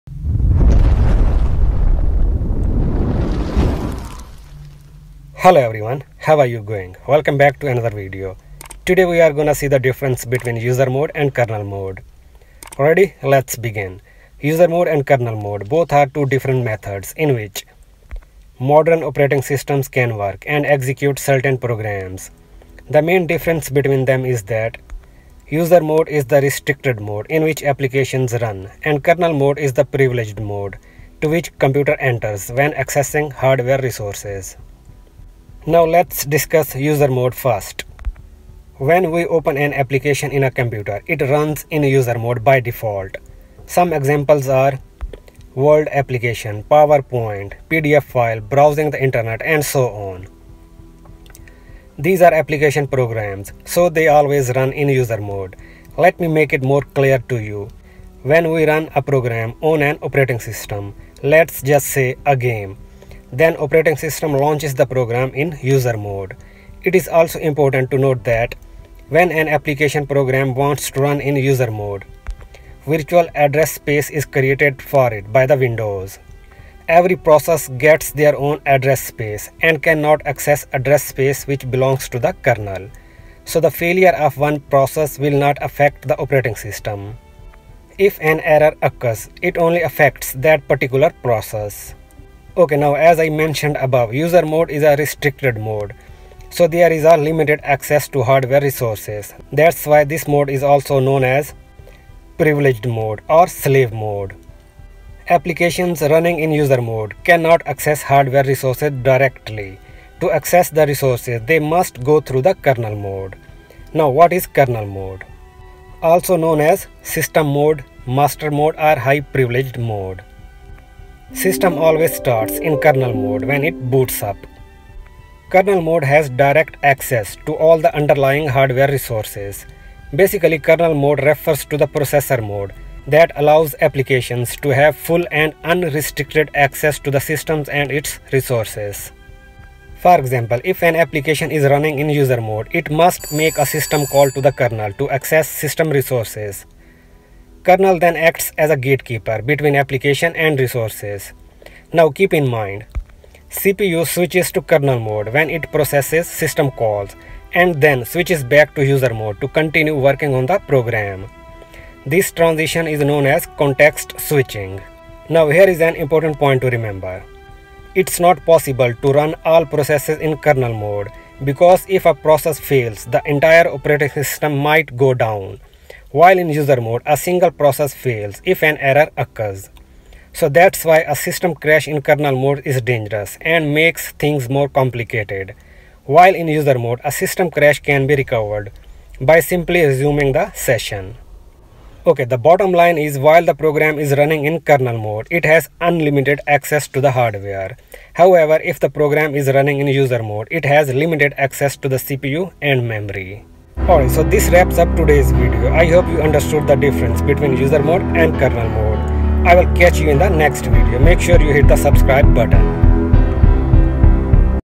User mode vs Kernel mode in operating system
This video is explaining the difference between user mode and kernel mode in the operating system. In this video, you will understand why user mode is restricted mode and kernel mode is unrestricted mode

Here is a brief overview
User mode and kernel mode are distinct privilege levels in computer systems. User mode is where regular applications and user-level processes run, and they have limited access to system resources and hardware. Kernel mode, on the other hand, is a higher privilege level where the operating system's core functions and critical processes reside. Kernel mode has unrestricted access to hardware and system resources, enabling it to manage memory, hardware devices, and system services. Transitioning between these modes allows the operating system to maintain security and stability by preventing user-level processes from interfering with critical system operations.